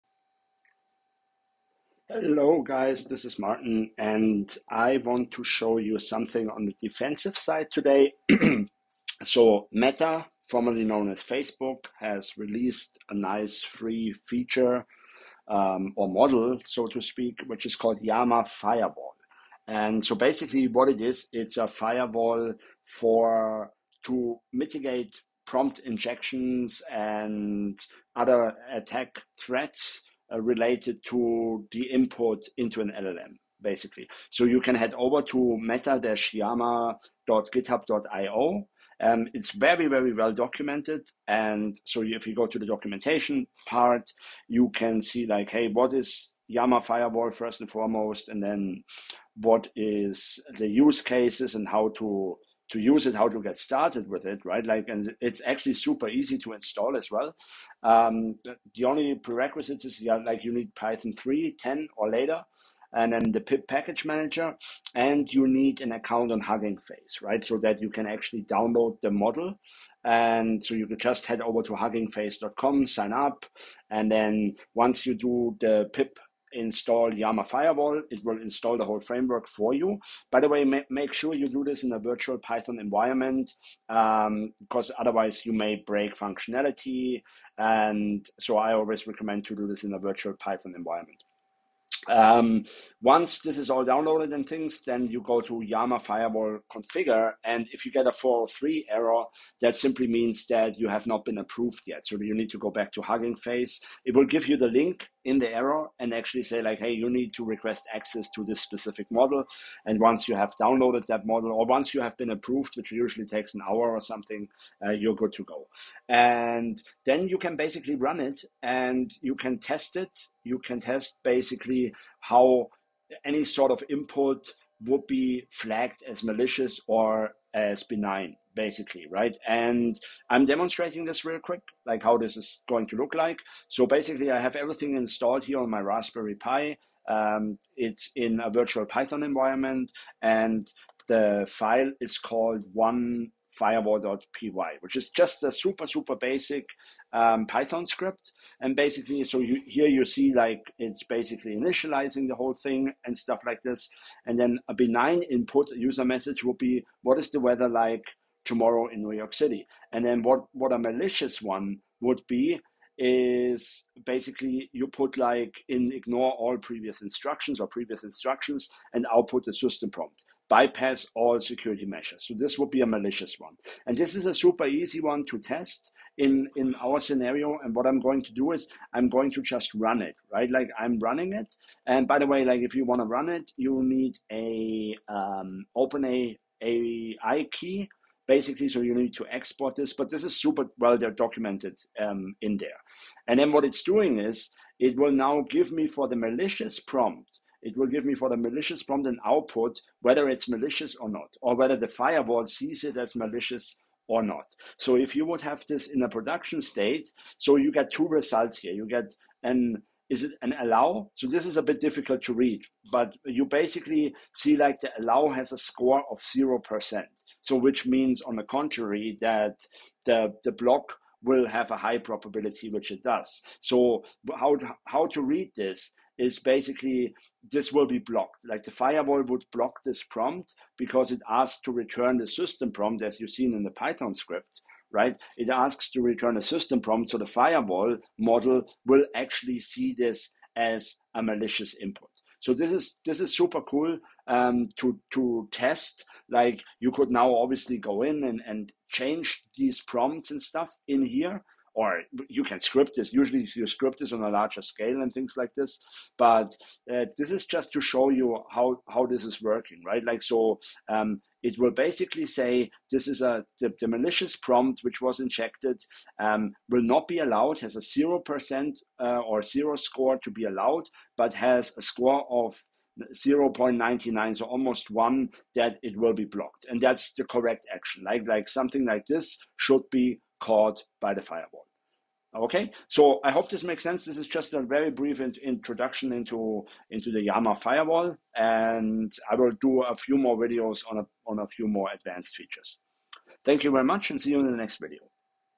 Meta Llama Firewall - Input guardrails for LLMs (Demo 1)
A quick video introducing Meta Llama Firewall - Input guardrails for LLMs (Demo 1)

💡 Ready to master AI security? Spots fill fast—save your seat now!